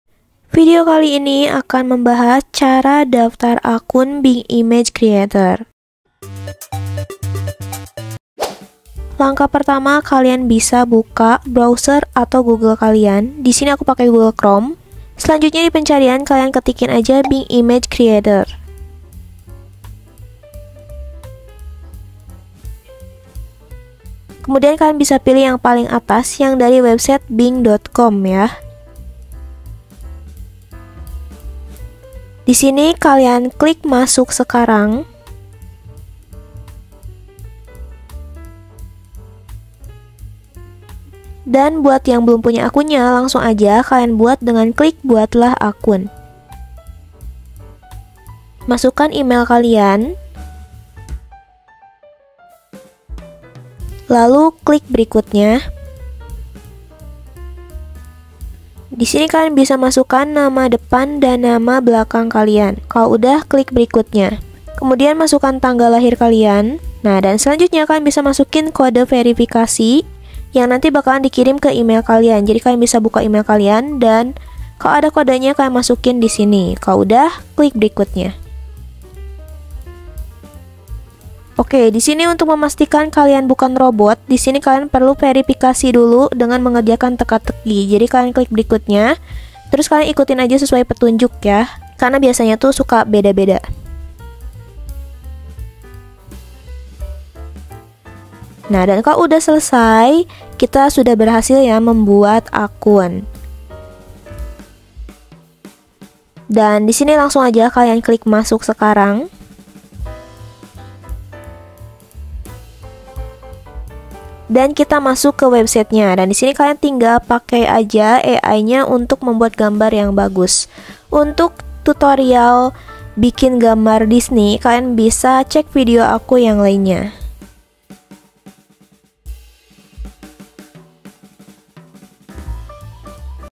Cara Daftar Akun Bing Image Creator Dengan Mudah
Untuk bisa bikin foto kartun disney kalian perlu daftar akun di bing image creator, untuk tutorialnya bisa kalian lihat di video kali ini. Selain itu kalian juga bisa lihat tutorial Cara menggunakan bing image creator disini:

Dan jika kalian ingin mengetahui berbagai tips tutorial berguna lainnya kalian bisa cek video-video lainnya di channel ini.